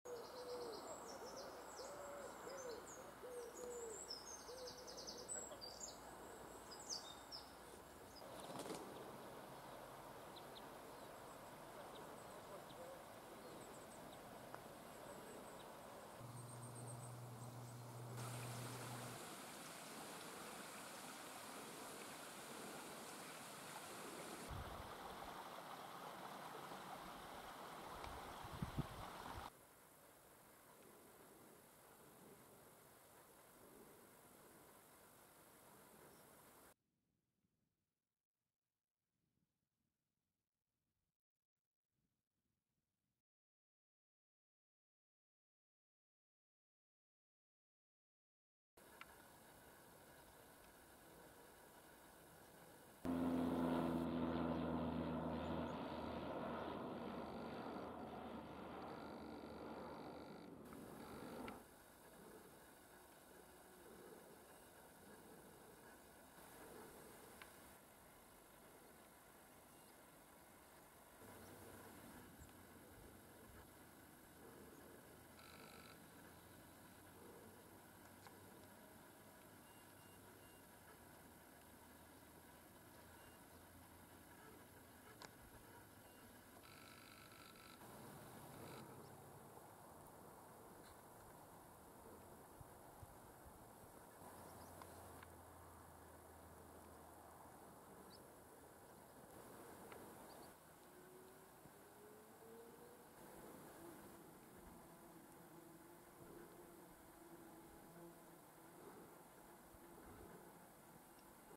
Willie's View: In The Menteith Hills
Our travel writer, Willie Shand, has been out and about in the Trossachs this week.